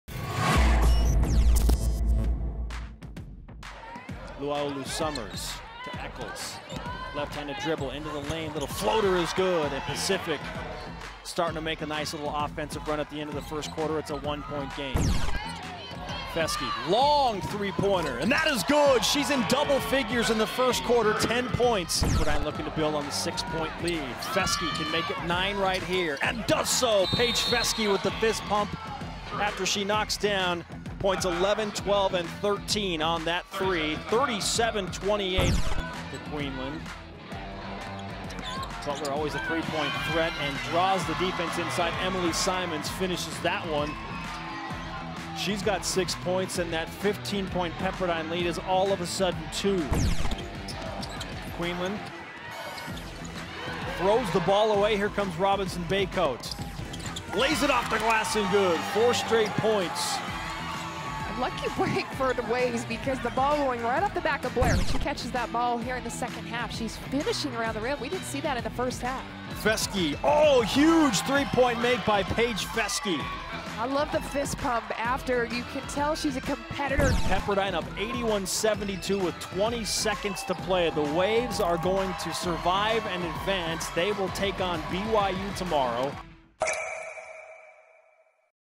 WCC Women's Basketball | Pepperdine vs. Pacific Highlights
The 9th-seeded Pepperdine Waves beat the 8th-seeded Pacific Tigers in the first round of the West Coast Conference Women's Basketball Championship.